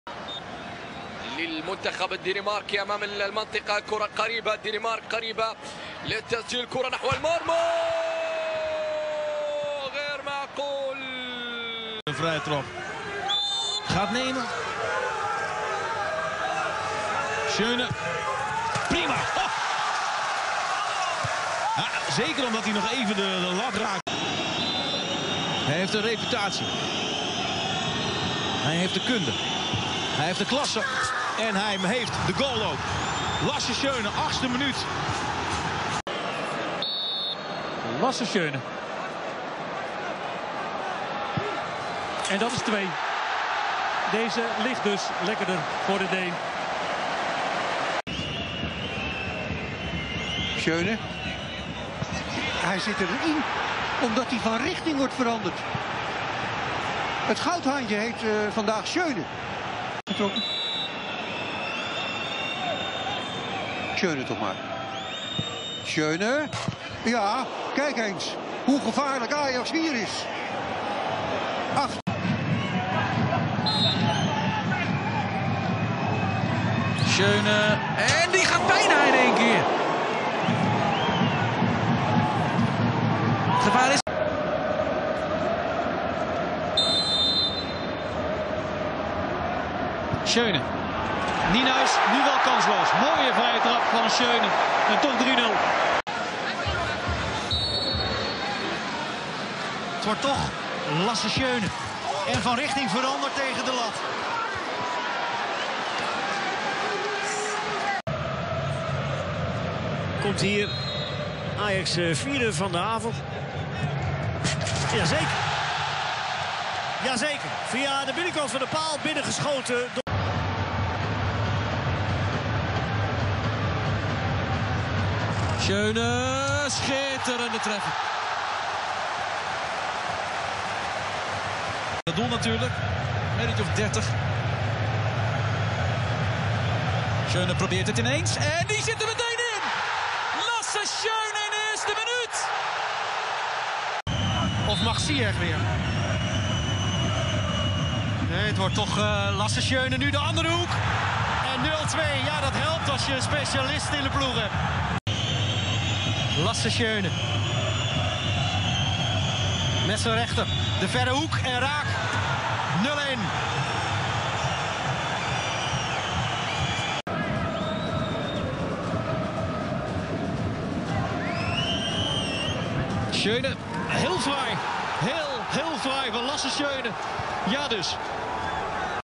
Lasse Schone - Ajax Amsterdam - Direct free kicks - Faltas directas
⚽ Recopilación de algunas de las faltas directas lanzadas por Lasse Schone con el Ajax de Amsterdam y la Selección de Dinamarca entre 2014 y 2019.